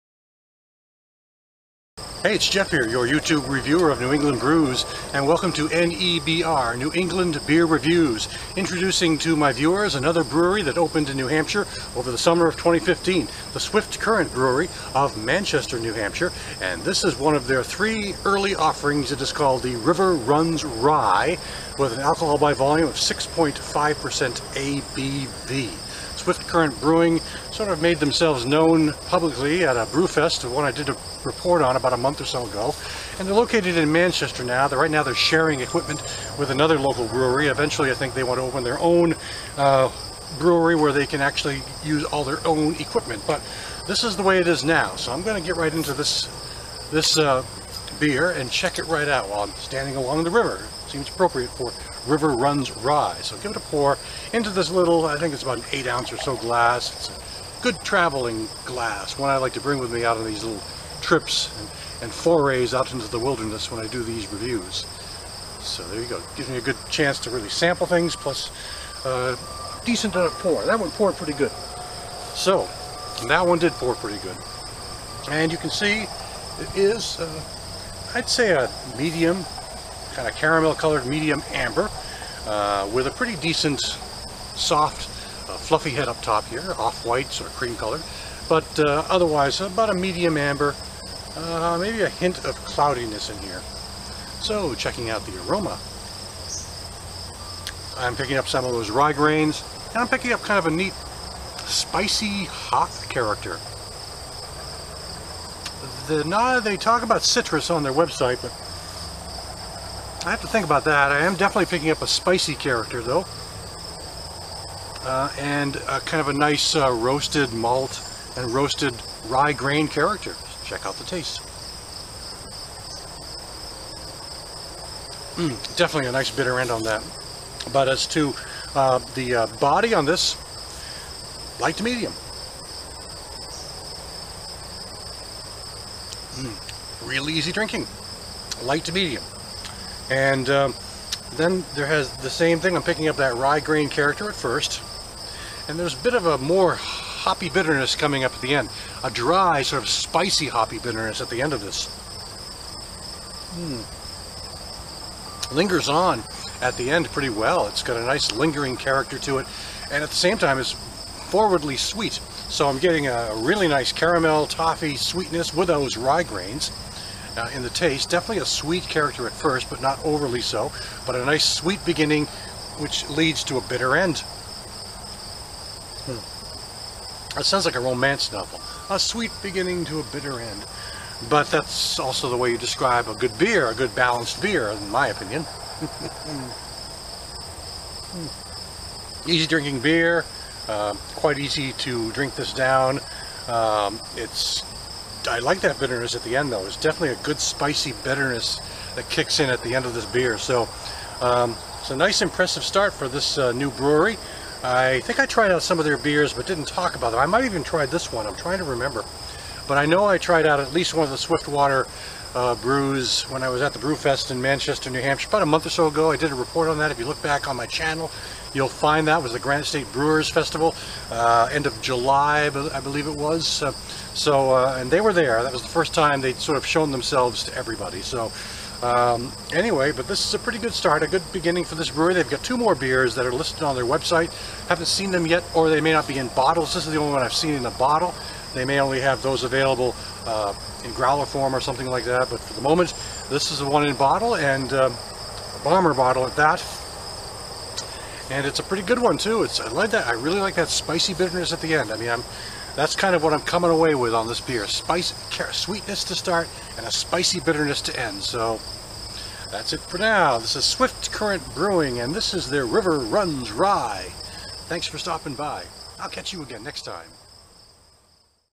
NEBR - Swift Current River Runs Rye
Greetings!  I'm an amateur reviewer of beers and ales from New England and today I am talking about a beer from one of New Hampshire's newest breweries - Swift Current Brewing of Manchester. Thank you for visiting my channel ad especially if you searched I out I look forward you comments